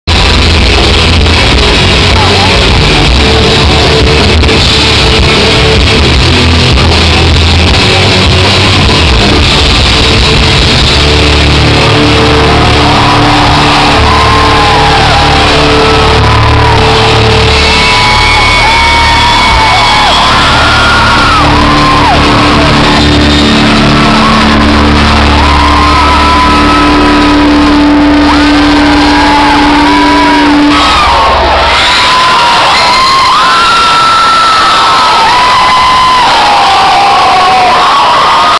AAR having a freestyle session
ending of their freestyle session
3/6/09 pipeline cafe, honolulu, hawaii